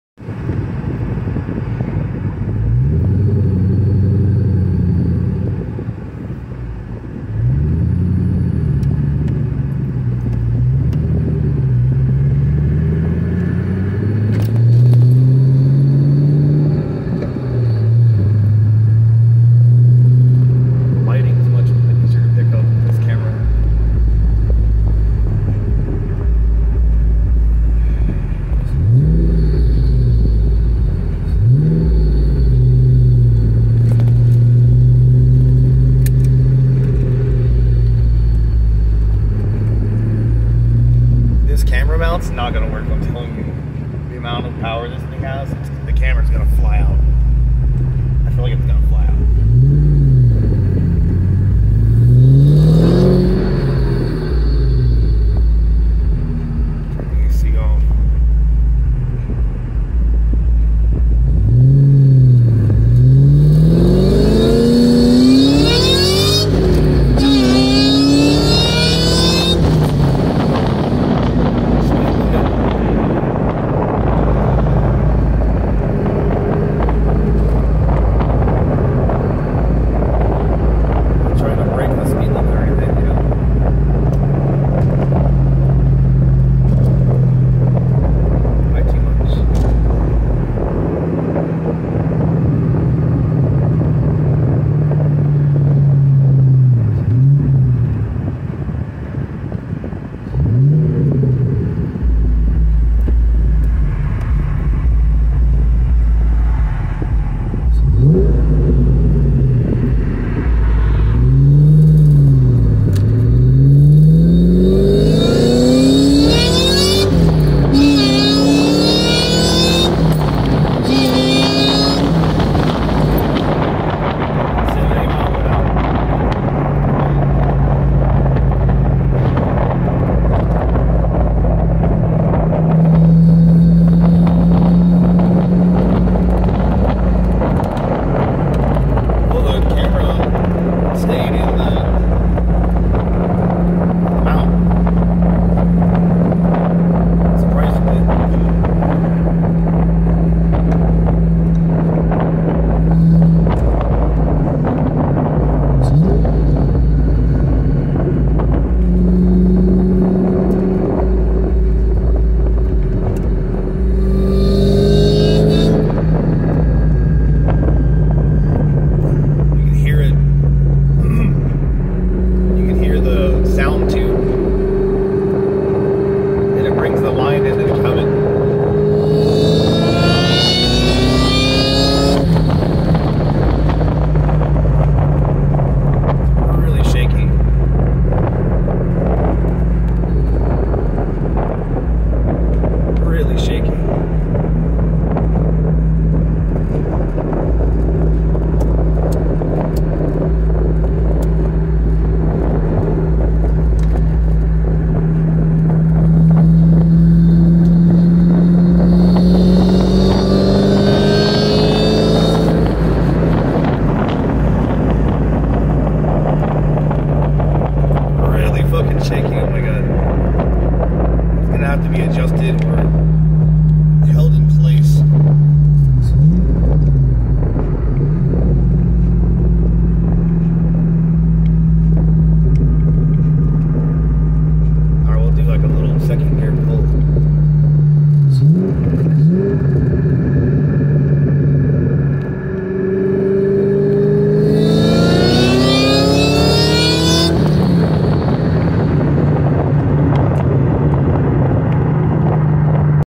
2003 Terminator mustang pulls test video.
2003 terminator mustang pulls. Just goofing around. 😅👌